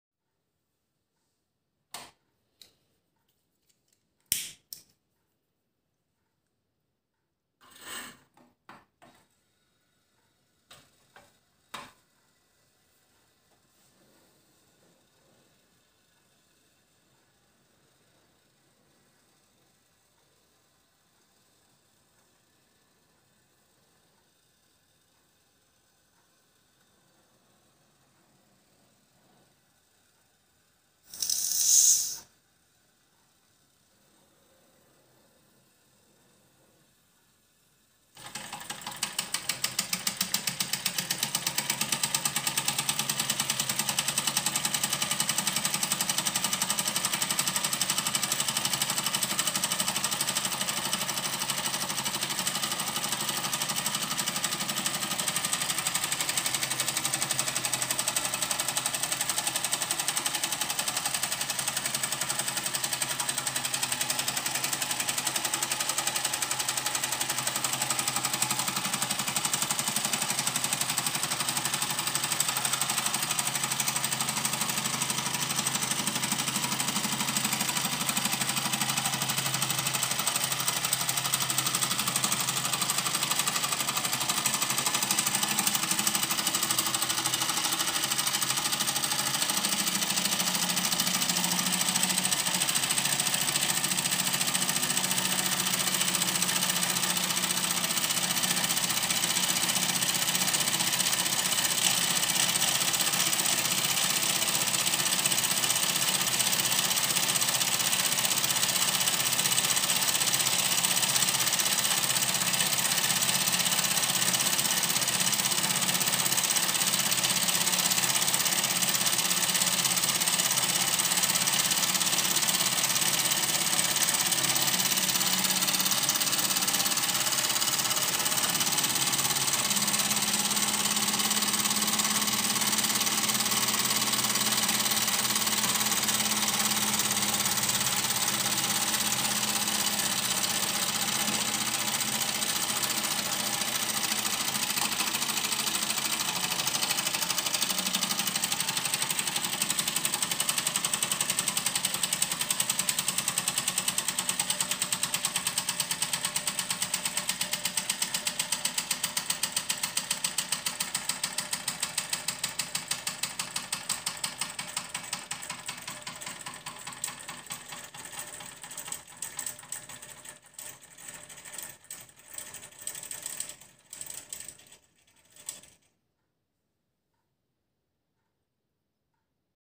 Fleischmann Dampfmaschine 125/2, Steam Engine Fleischmann 125/2
Die Restaurierung dieser kleinen Fleischmann-Dampfmaschine vom Typ 125/2 von 1954 beschränkte sich lediglich auf den Austausch von drei Dichtungen und das äußerliche Putzen des Kessels. Ansonsten verblieb sie im Originalzustand und behielt ihre authentische Patina. Obwohl der originale Esbit-Brenner noch vorhanden ist, wird sie hier mit einem passenden Fleischmann-Spiritusbrenner Nr. D-195 befeuert. Ein quirliges kleines Maschinchen aus einer Zeit, als Wünsche nach technischem Spielzeug in den meisten Fällen noch bescheidener ausfielen.